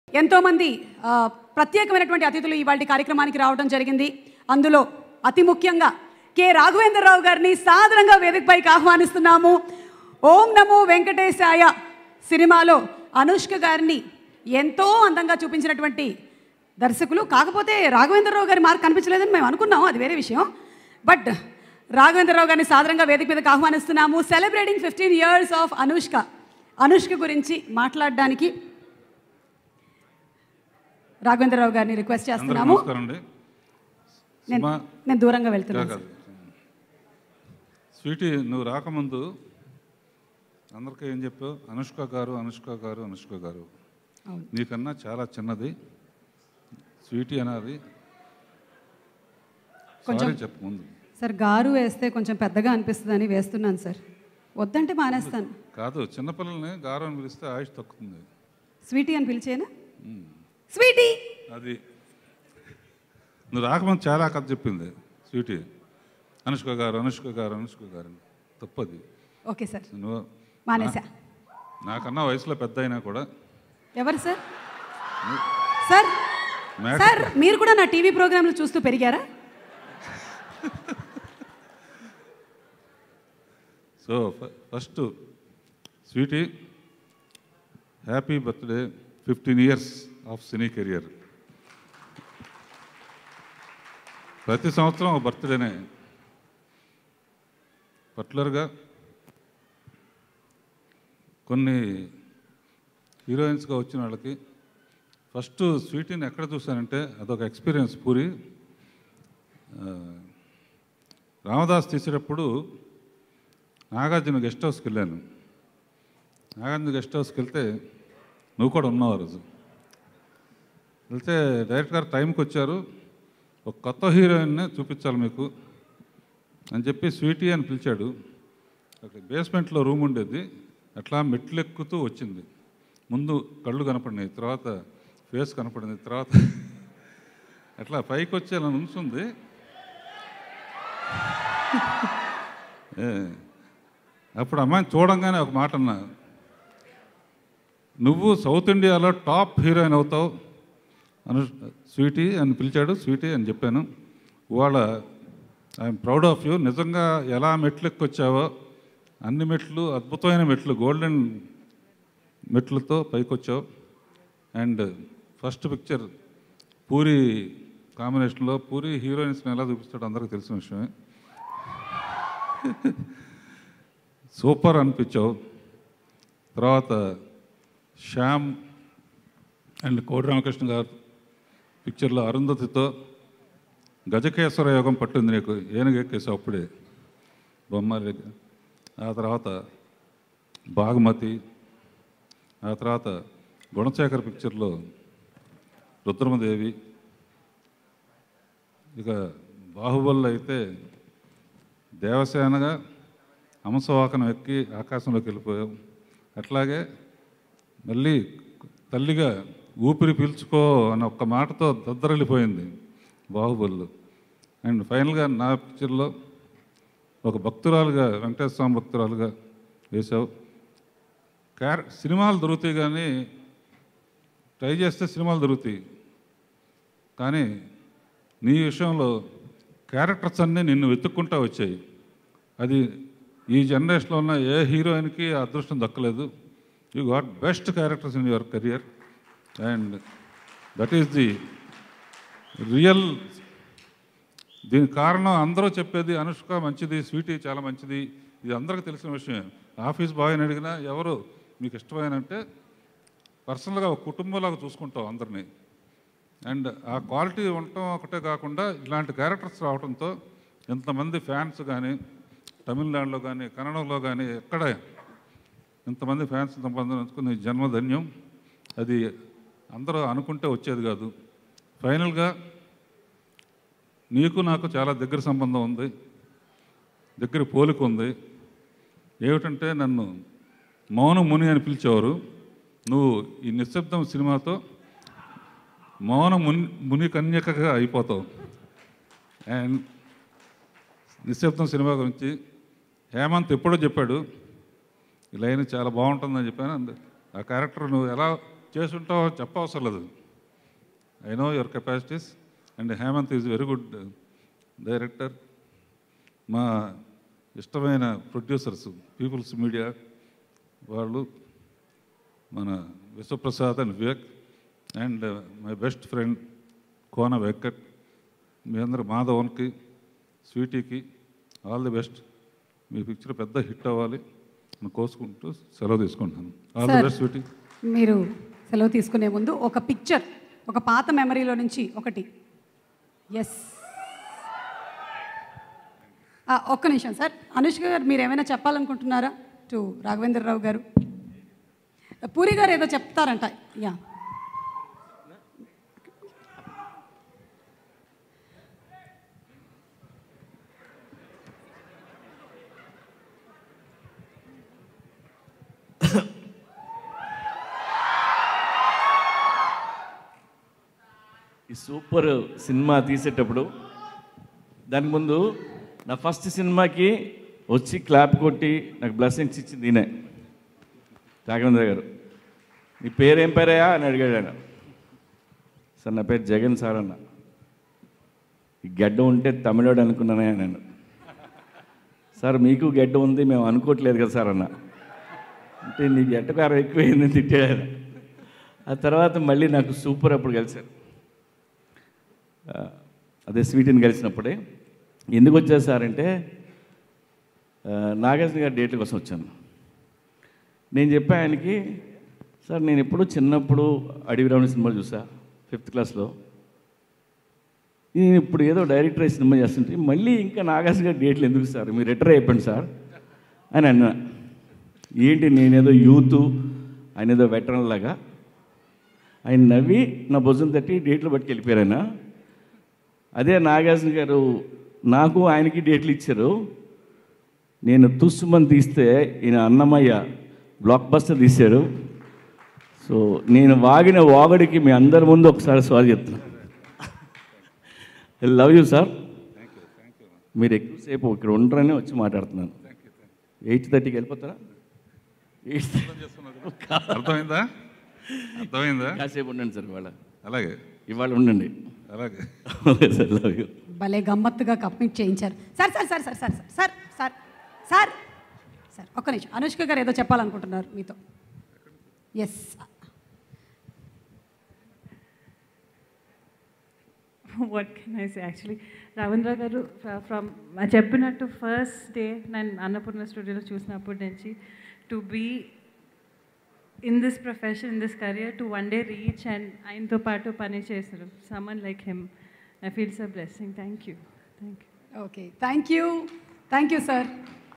K Raghavendra Rao Speech @ 15 years of AnushkaShetty Celebrations
Watch K Raghavendra Rao Speech @ 15 years of AnushkaShetty Celebrations